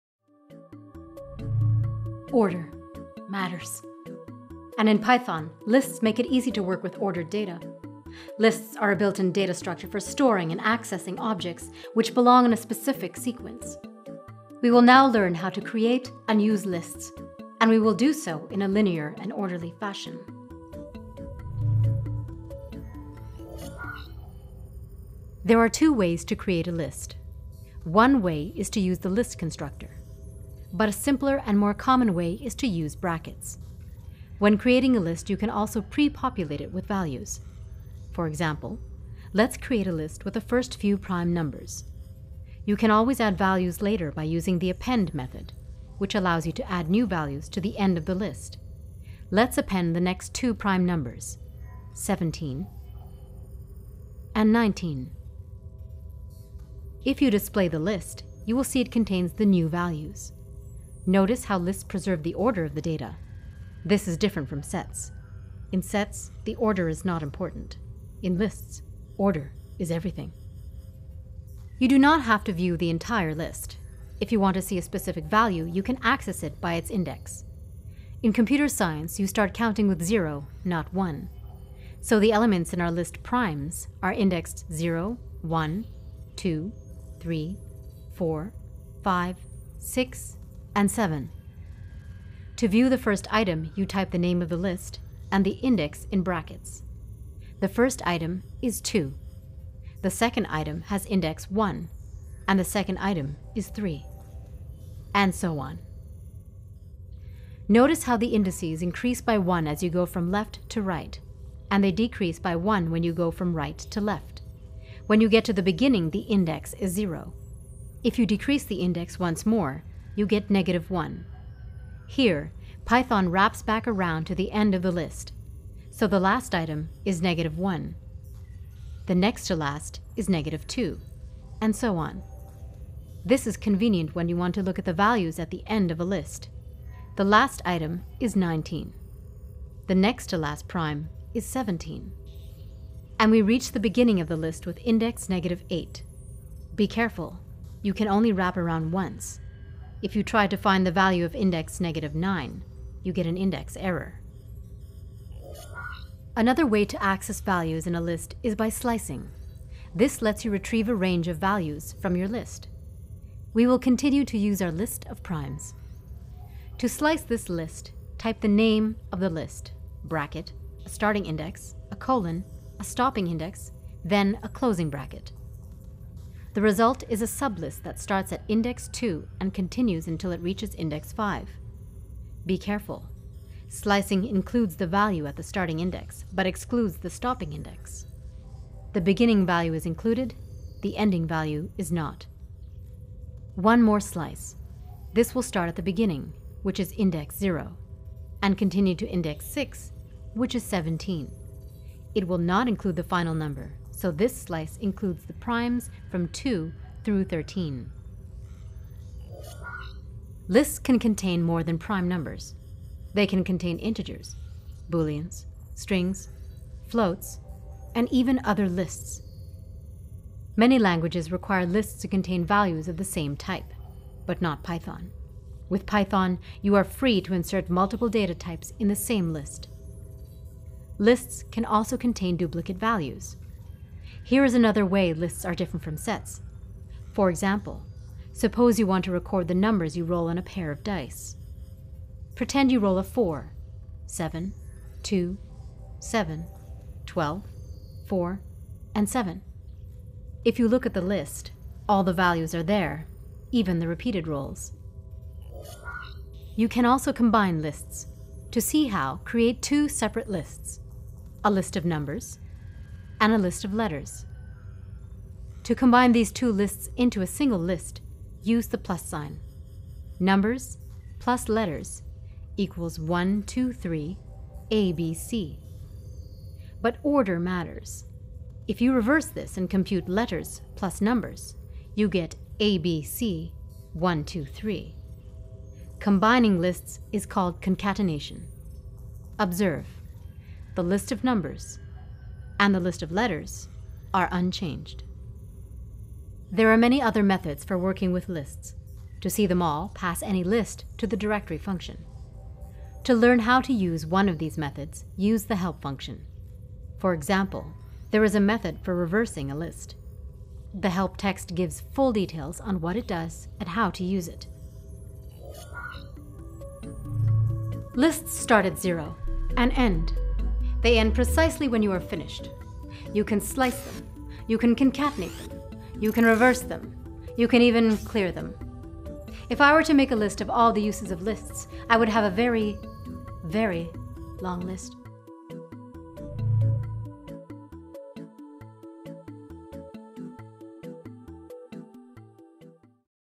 Python Lists || Python Tutorial || Learn Python Programming
𝙎𝙩𝙖𝙮 𝙞𝙣 𝙩𝙝𝙚 𝙡𝙤𝙤𝙥 𝙄𝙉𝙁𝙄𝙉𝙄𝙏𝙀𝙇𝙔 Lists are a way to store ordered data.  In this Python tutorial, we show you how to create lists, access elements by index, slice lists, join two lists (concatenation), and more.  We will talk about sets, dictionaries and tuples in separate videos.

We recommend:
Python Cookbook, Third edition from O’Reilly

The Mythical Man Month - Essays on Software Engineering & Project Management

Python instructor: Ulka Simone Mohanty
Written & Produced by Michael Harrison
FX by Andriy Kostyuk